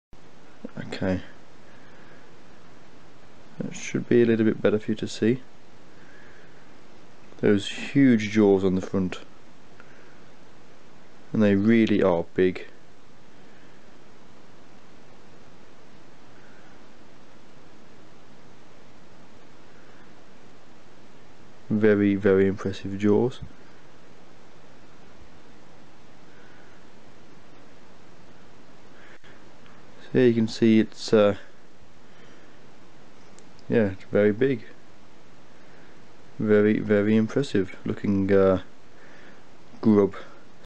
A face only a mother could love (Beetle Grub)
This is the Larva (Grub) of a Rhino Beetle (Allomyrina dichotoma). It hardly has the charm of a pug.